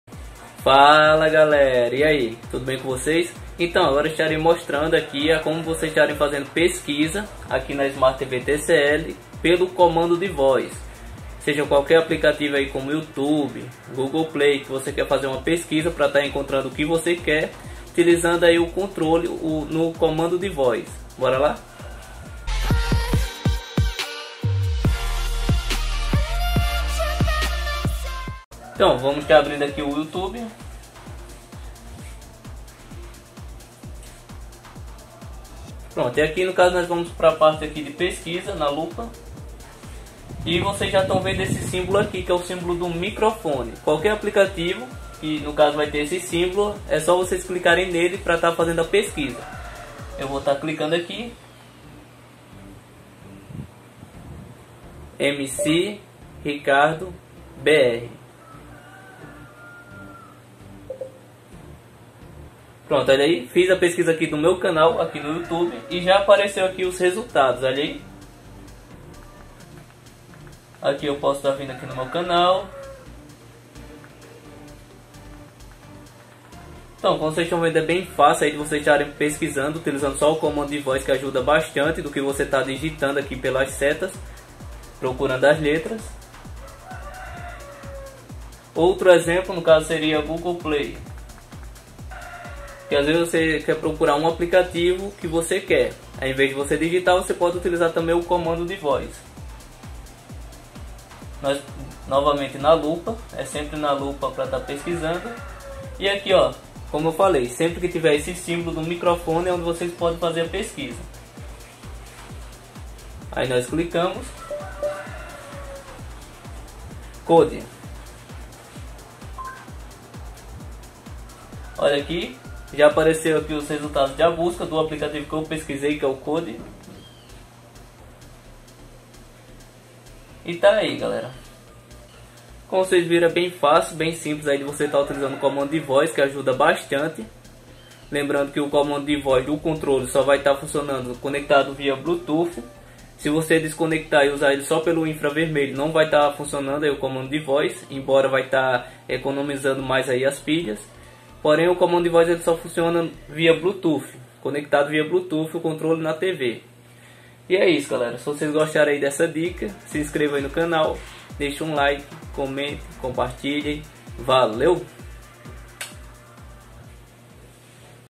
Como fazer pesquisa pelo COMANDO DE VOZ na SMART TV TCL
Fala galera, agora venho mostrar como fazer pesquisas pelo comando de voz na smart tv tcl.

⚠️Teste o Amazon Prime Vídeo Grátis por 30 dias e assista vários filmes e séries